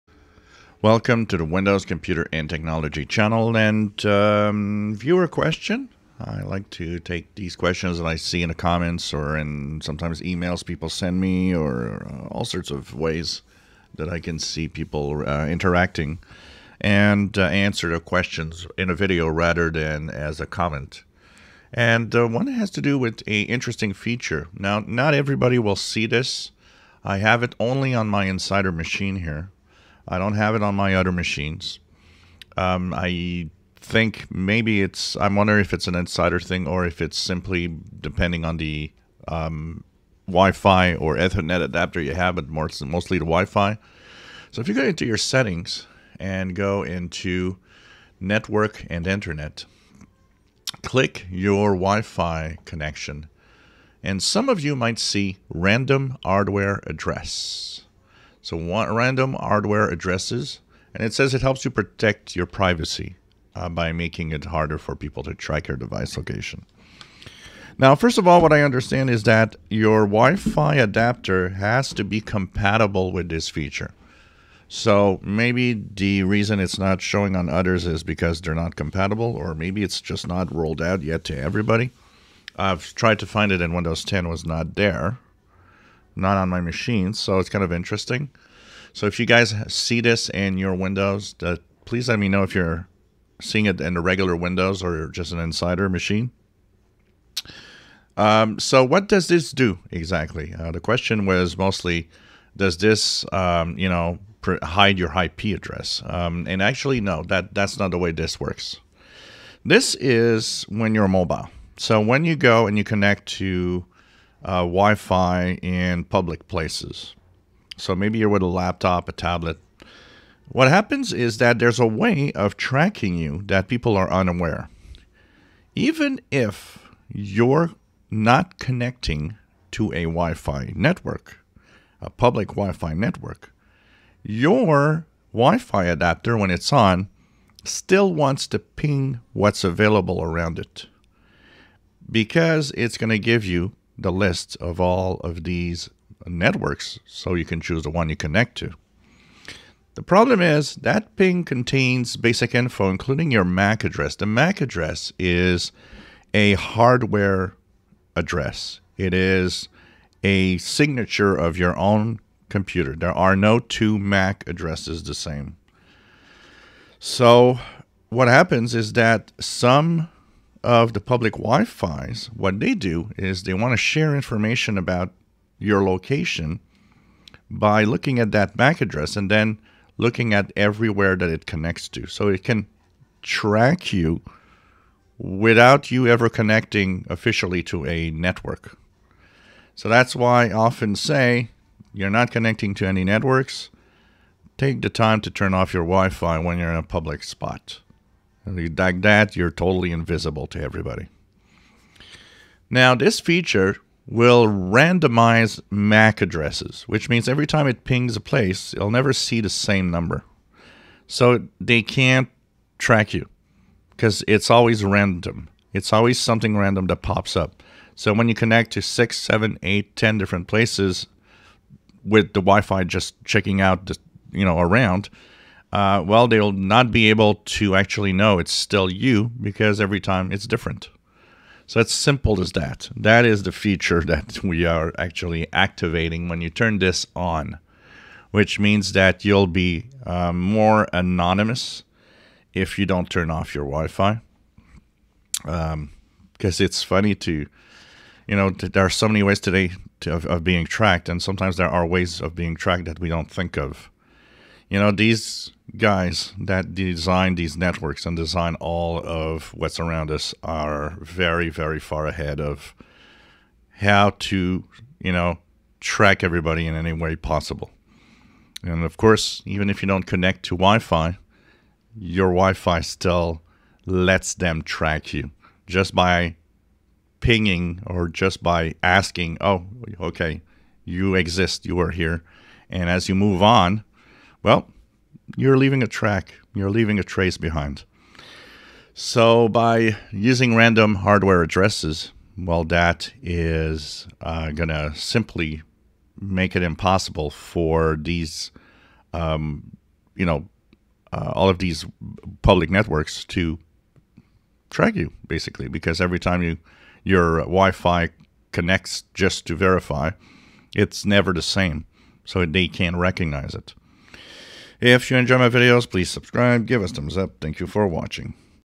What is Random Hardware Addresses in Wi-Fi settings in Windows
This is a very unique feature when you use your PC on public networks